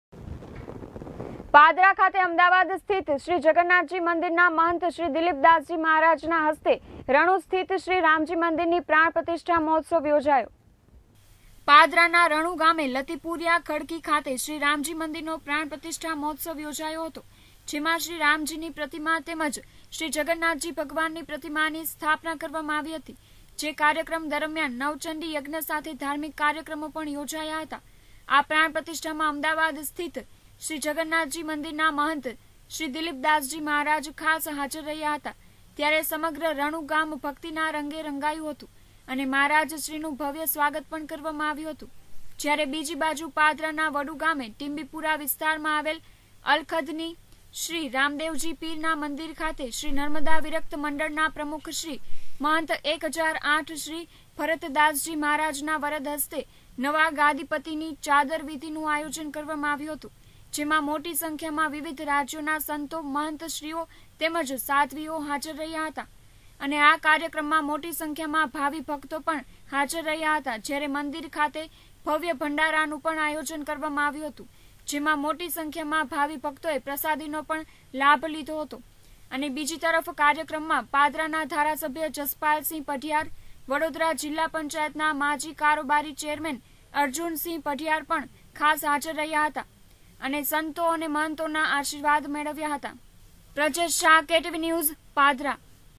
PADRA: પાદરા ખાતે અમદાવાદ સ્થિત શ્રીજગન્નાથજી મંદિરના મહંત શ્રીદિલીપદાસજી
PADRA: પાદરા ખાતે અમદાવાદ સ્થિત શ્રીજગન્નાથજી મંદિરના મહંત શ્રીદિલીપદાસજી મહારાજના હસ્તે રણું સ્થિત શ્રીરામજી મંદિરની પ્રાણ પ્રતિષ્ઠા મહોત્સવ યોજાયો.